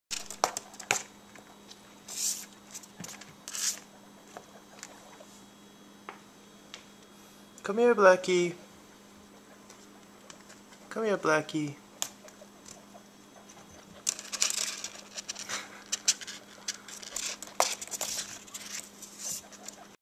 Come here, Blackie!
So cute to watch their little bodies try to climb up the wall haha. (We still haven't confirmed their genders yet, so "Blackie" is still what we call the black one for now.)

They are 16 days old...maybe we should try again to figure out their sex. It seems at three weeks, it becomes much easier to tell.